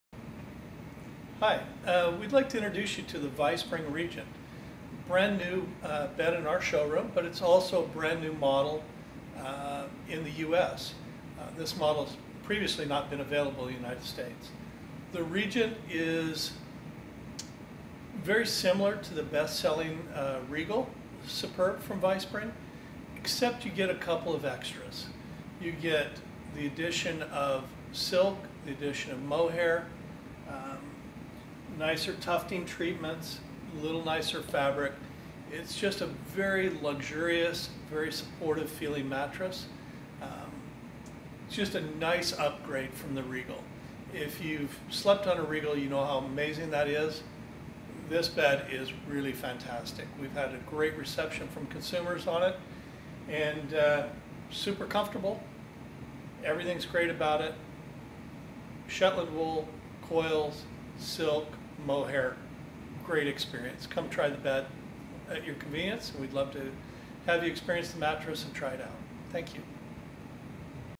Vispring Regent Overview from the sleep Store Santana Row San Jose
Get to know all about the Regent from Vispring with Barrie Brown at the sleep Store in San Jose's Santana Row.